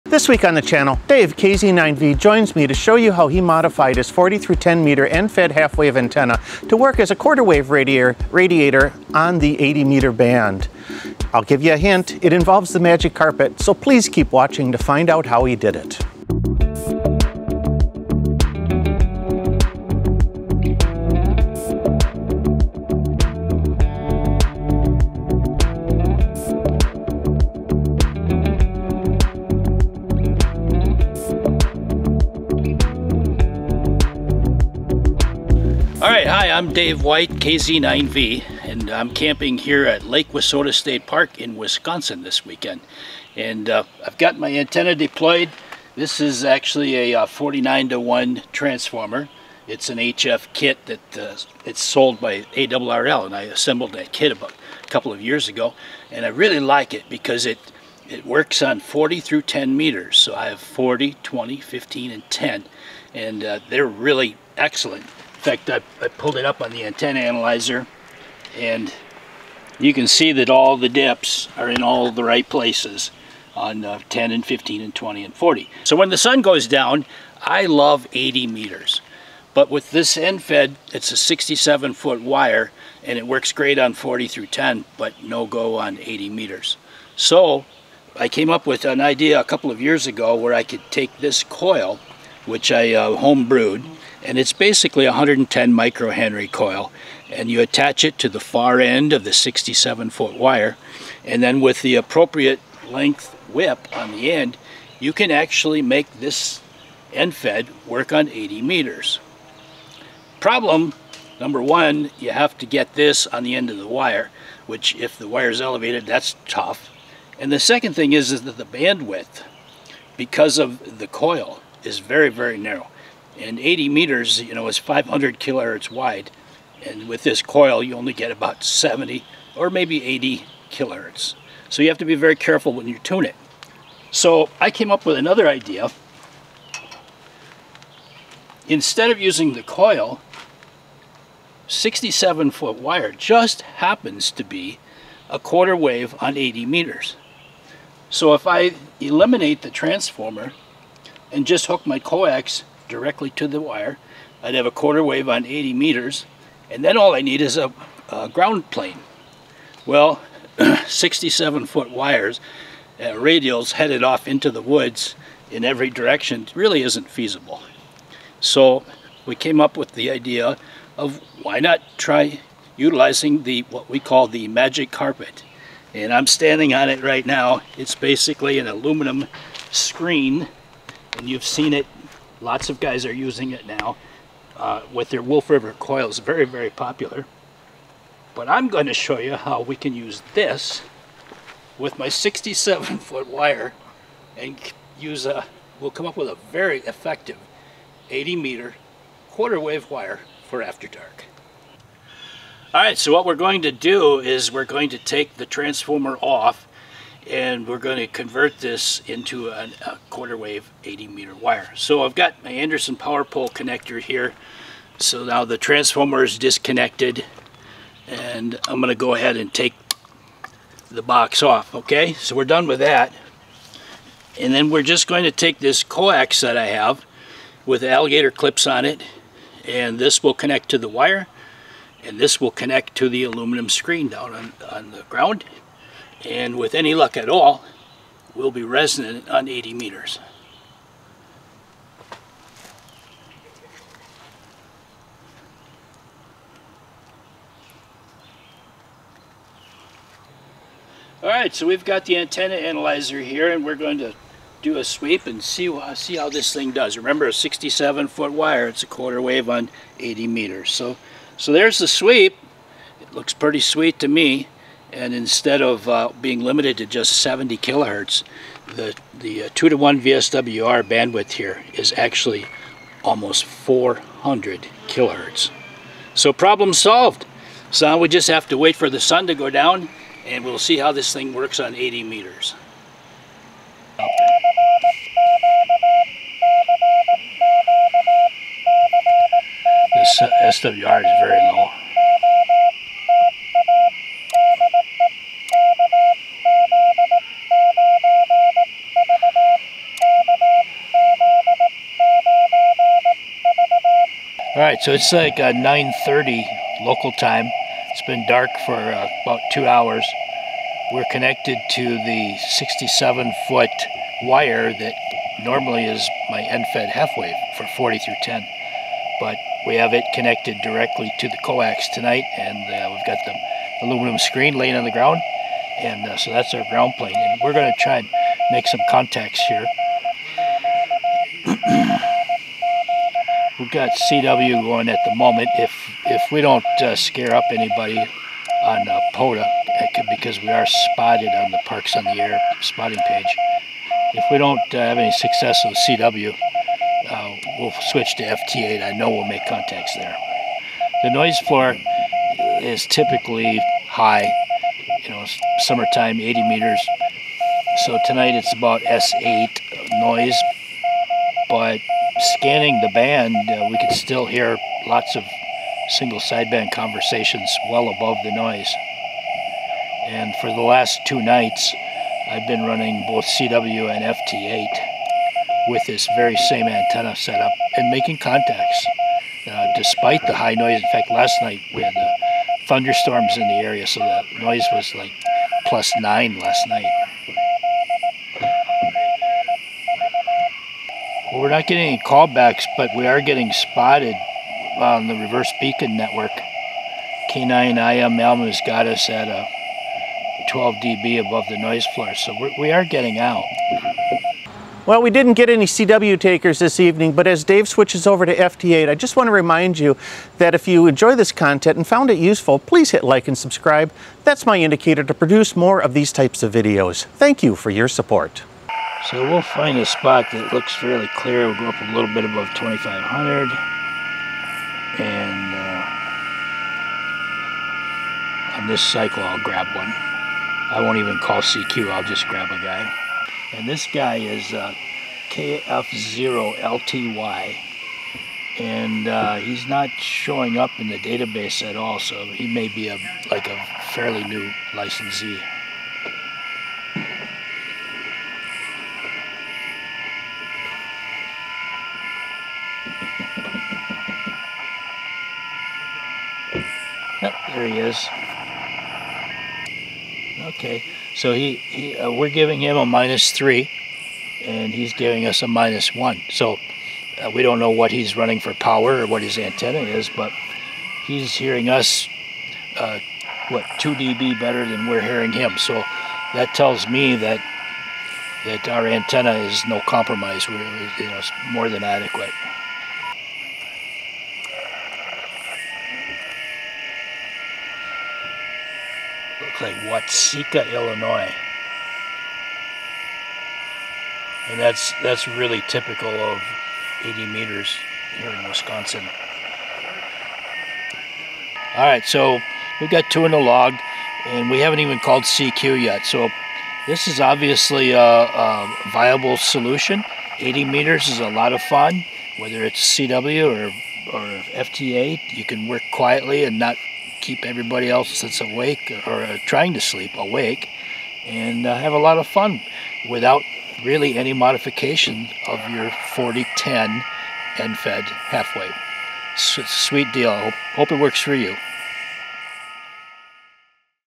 The Magic Carpet rides again! This time on 80 meters
This week, Dave, KZ9V, shares with us how he modified his 40 through 10 meter end fed half wave antenna to work as a ¼ wave radiator on 80 meters using the window screen ground.